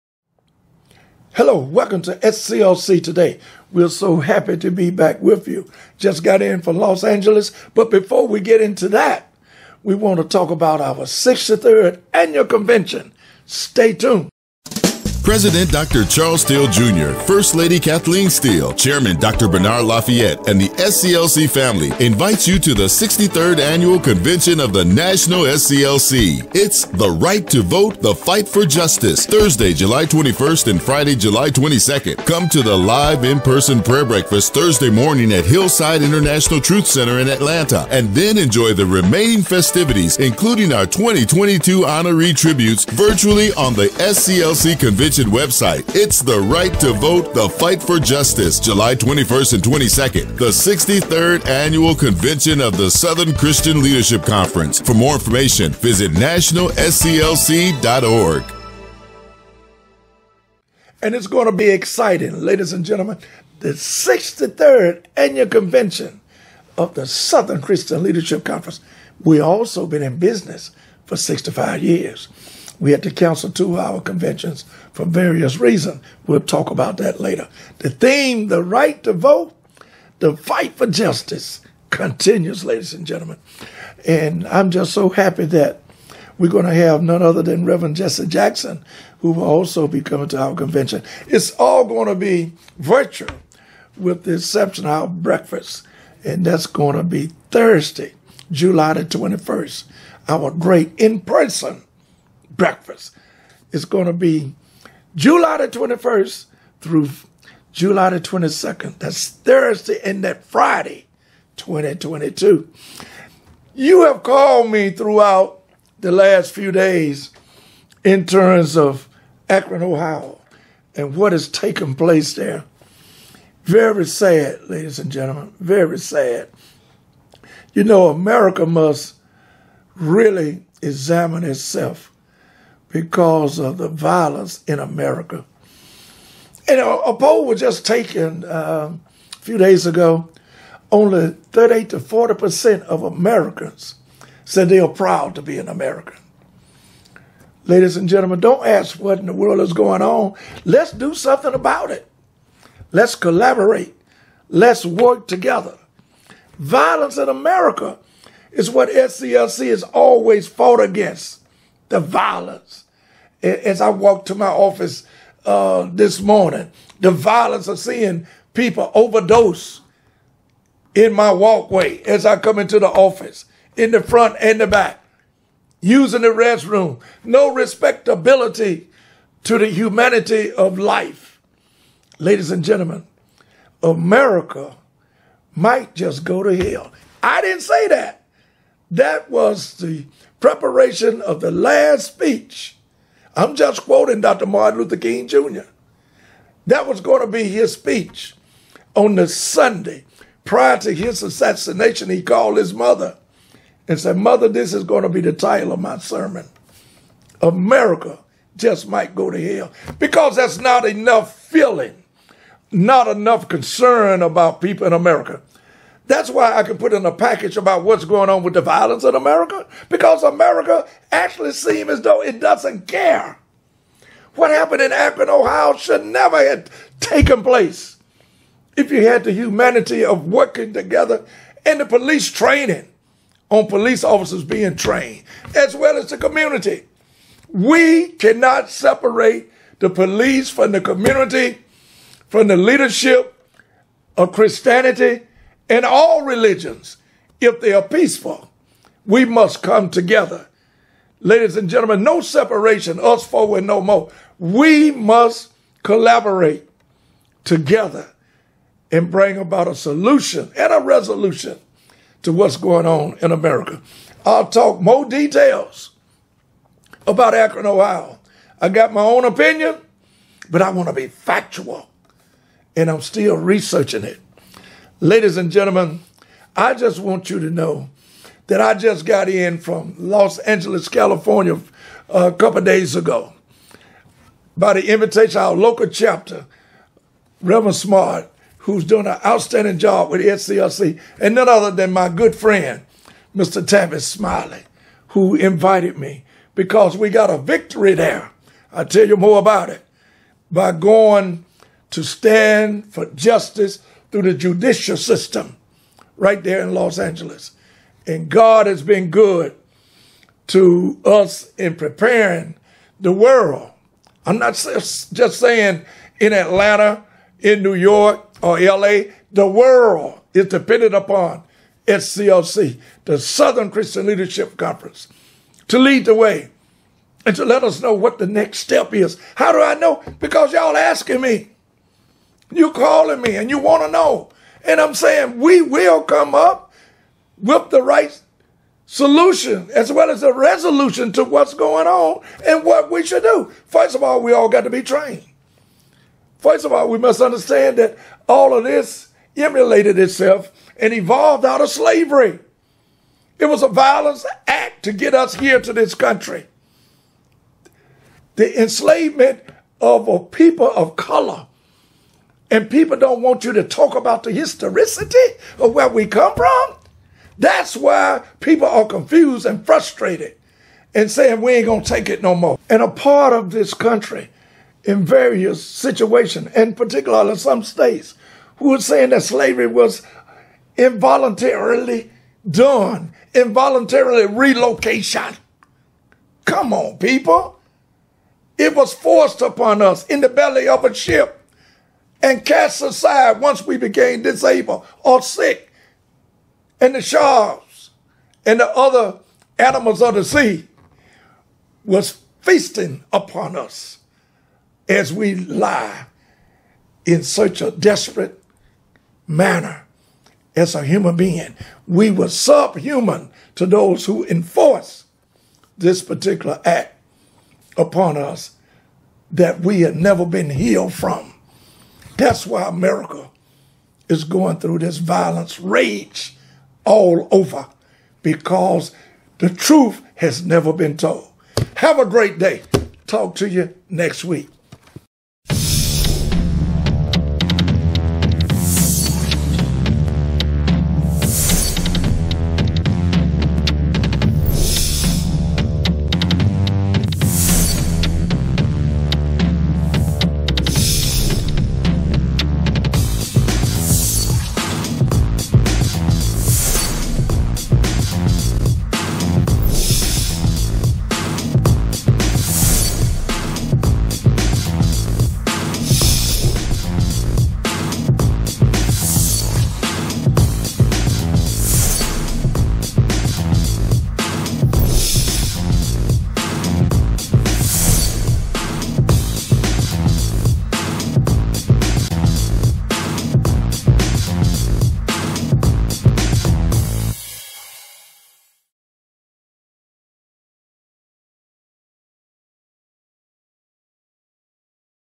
SCLC TODAY--July 7, 2022
SCLC President/CEO Dr. Charles Steele, Jr. highlights this month's Annual SCLC Convention coming up July 21-22!
Violence in America Must End!
Slavery or "Involuntary Relocation"...what do you think?